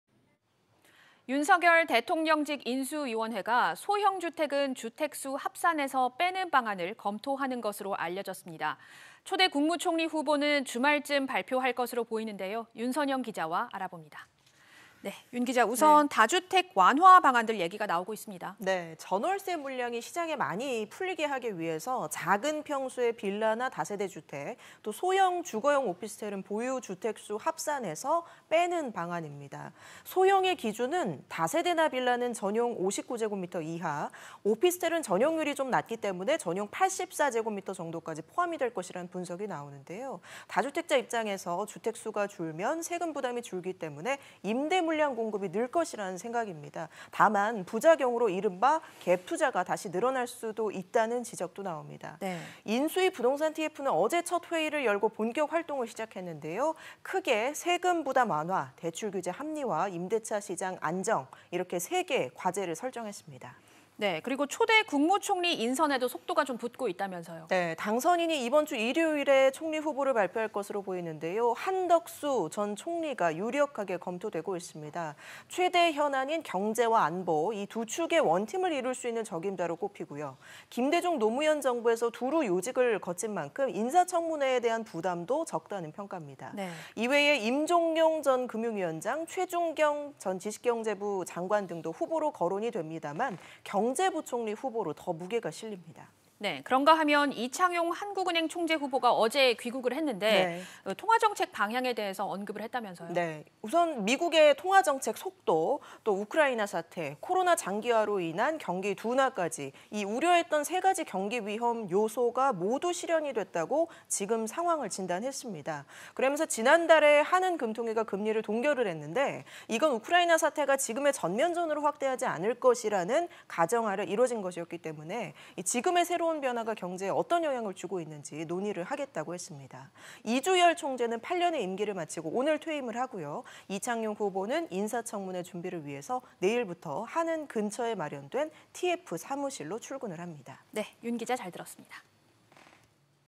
소형은 주택수 제외 검토…주말 총리 후보 발표
[앵커]

윤석열 대통령직 인수위원회가 소형주택은 주택 수 합산에서 빼는 방안을 검토하는 것으로 알려졌습니다.

초대 국무총리 후보는 주말쯤 발표할 것으로 보이는데요.

윤선영 기자와 알아봅니다.

우선 다주택 완화 방안들 얘기가 나오고 있네요?

[기자]

전월세 물량이 많이 나오게 하기 위해 작은 평수의 빌라나 다세대주택, 소형 주거용 오피스텔은 보유주택 수 합산에서 빼는 방안입니다.